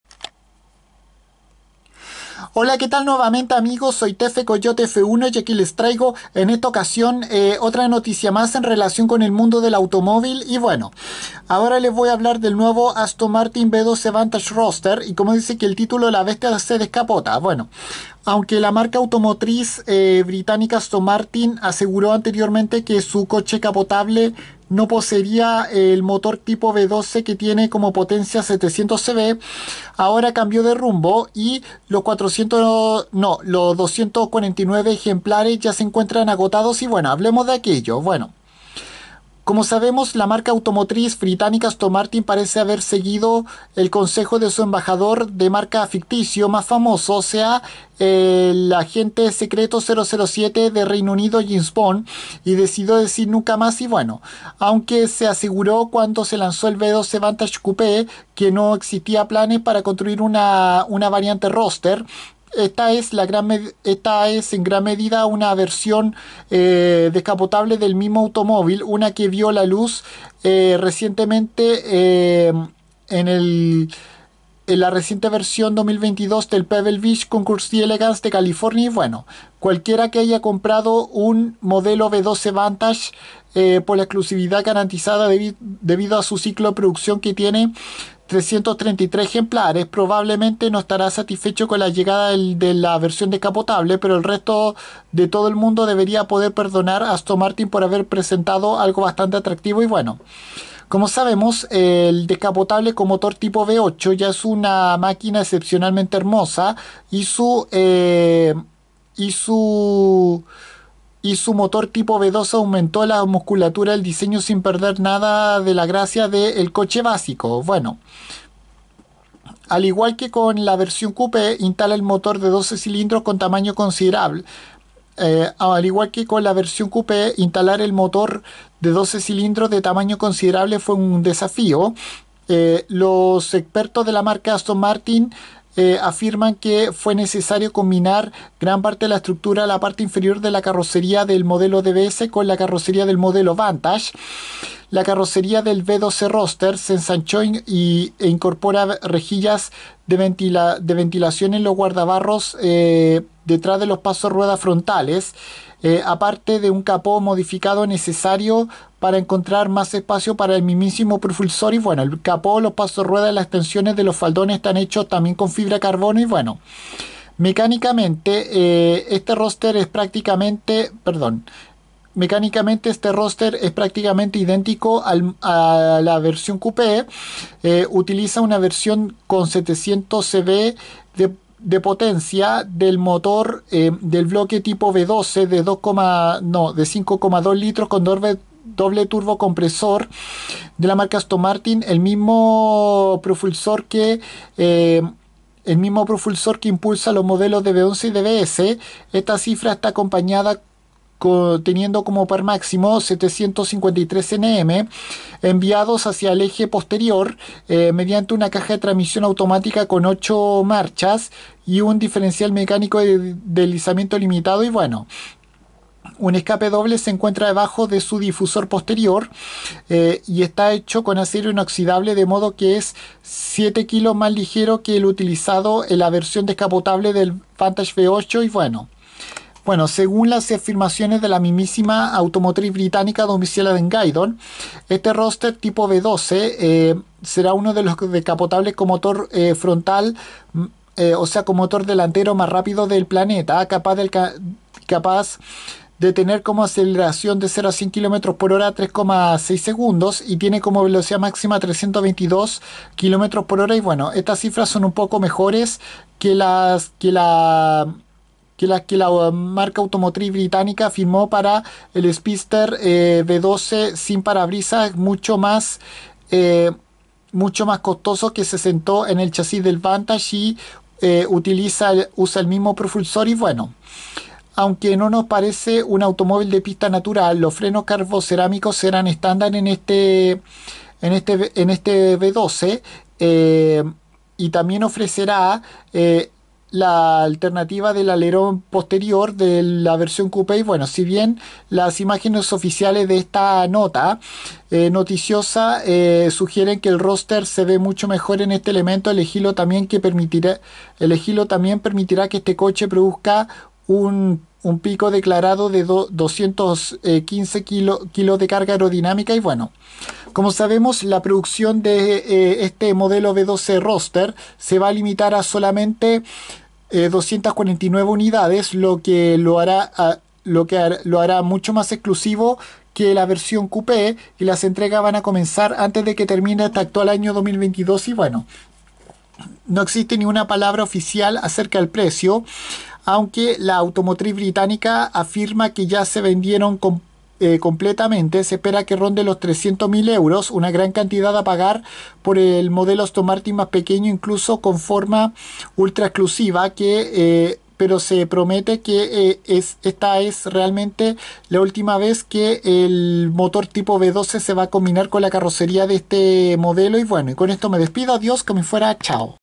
Aston Martin V12 Vantage Roadster información
En este vídeo les explico sobre esta exclusiva variante descapotable que aunque la marca automotriz británico aseguró que no poseería el mismo motor tipo V12 con potencia 700 CV ahora todos sus 249 ejemplares ya se encuentran vendidos